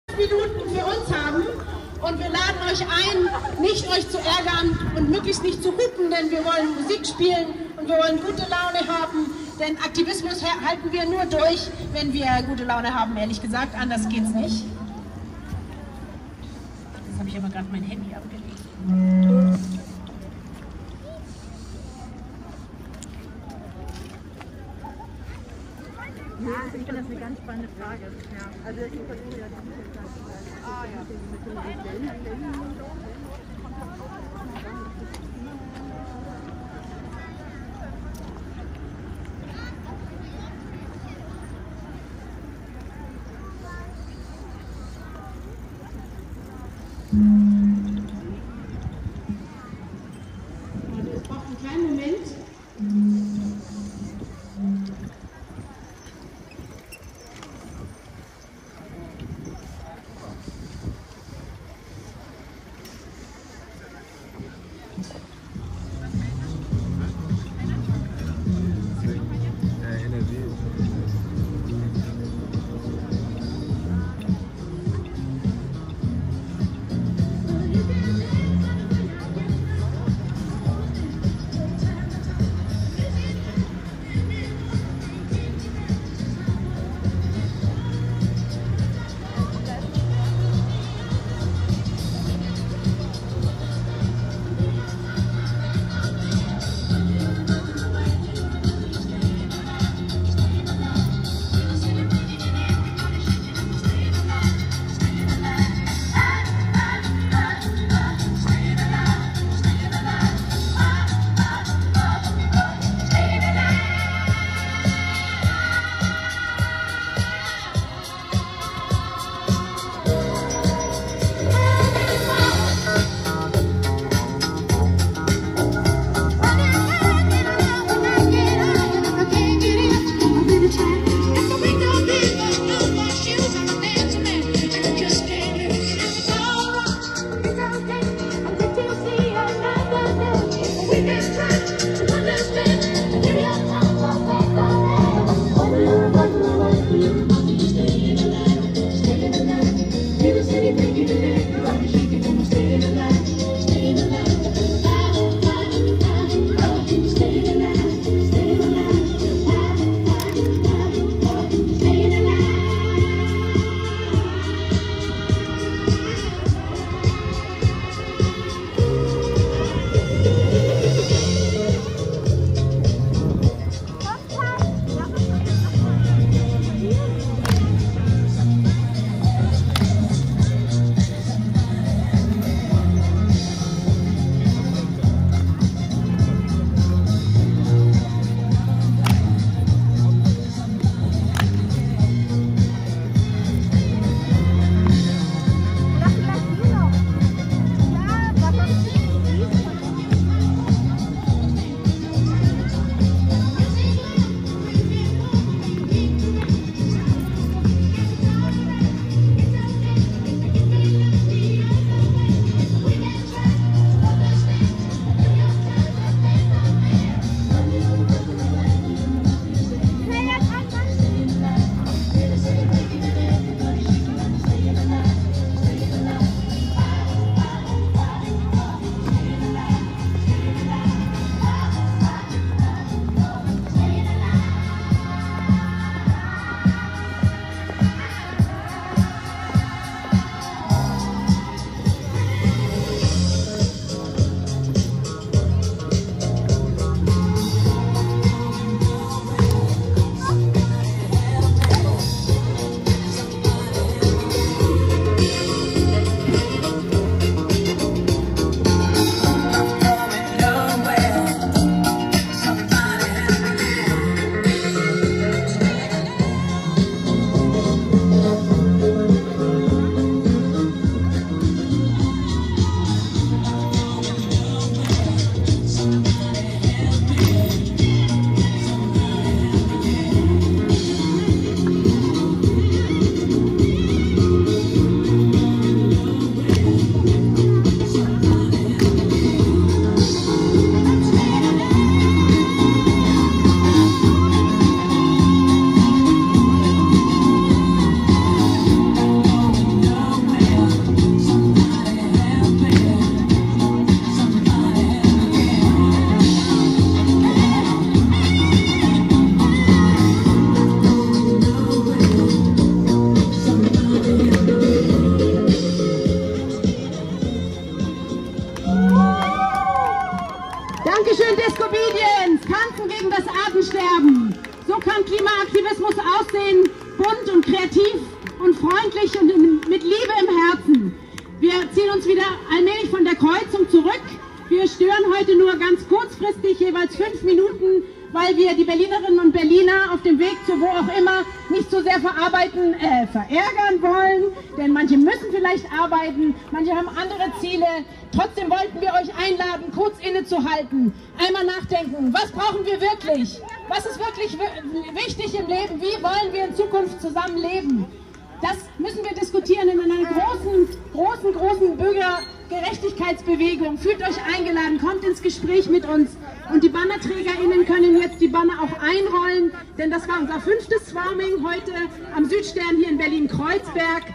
12.2.22 Swarming und Discobedience von Extinction Rebellion in Berlin
VALENTINS-SWARMING
BERLIN
Es ist SWARMING-Zeit!

WANN und WO? Samstag, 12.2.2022, 12.00 Uhr, Südstern, Kreuzberg, Berlin. Treffpunkt vor der U-Bahnstation nahe des Wochenmarktes.

WAS? Familien-Swarming: 5 x 5 Minuten tatkräftiger Widerstand auf der Straße gegen leere Versprechungen und scheinheilige Worthülsen.

Bannern, flyern, singen, tanzen, malen: Wir holen uns das Recht auf Umweltschutz - sofort und bunt.

Komm dazu, mach mit! Unser Swarming ist angemeldet, Corona-konform und kinderkompatibel!

ENGLISH

It's SWARMING time! :

WHEN and WHERE? Saturday, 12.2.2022, 12.00 noon, Südstern, Kreuzberg, Berlin. Meeting point in front of the underground station near the weekly market.

WHAT? Family-swarming: 5 x 5 minutes of energetic resistance on the streets against empty promises and hypocritical empty words.

Banner, flyer, singing, dancing, painting: We demand the right to environmental protection - immediately and colourfully.

Join in! Our swarming is registered, corona-compliant and child-friendly!